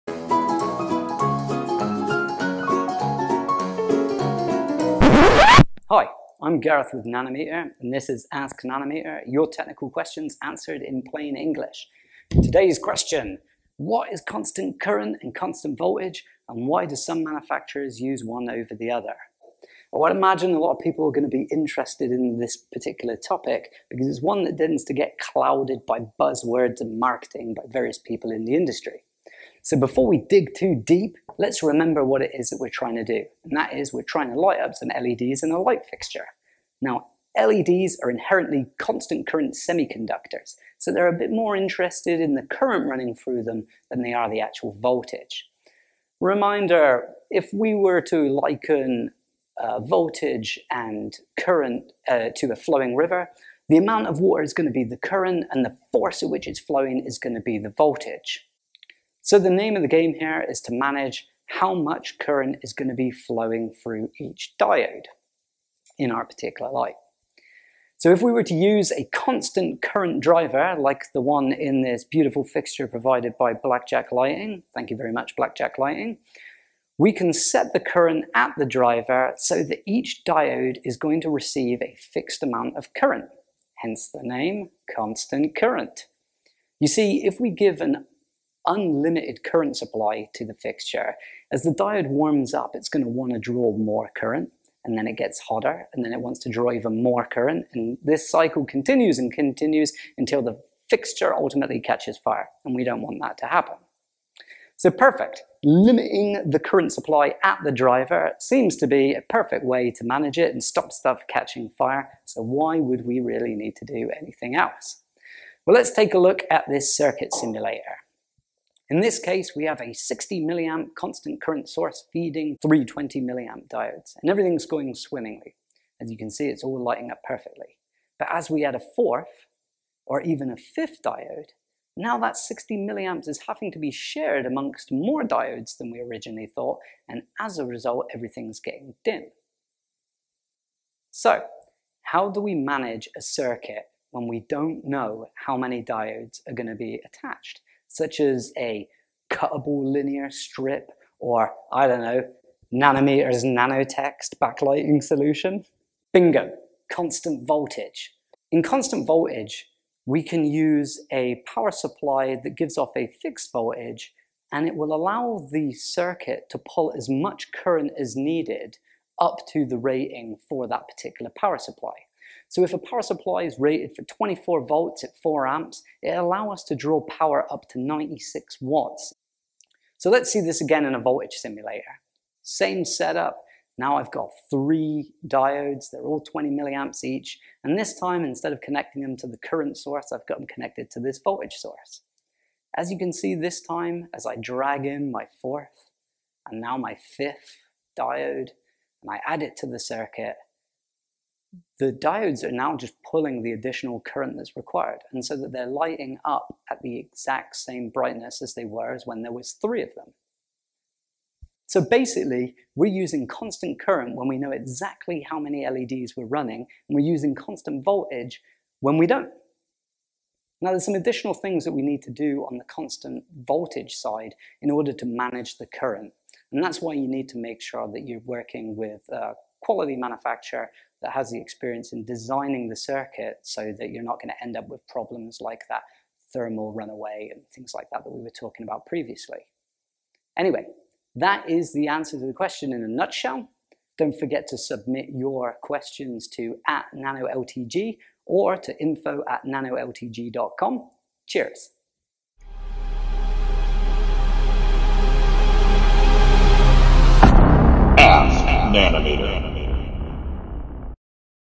Constant Current vs. Constant Voltage | Ask Nanometer
- What is the difference between constant current versus constant voltage?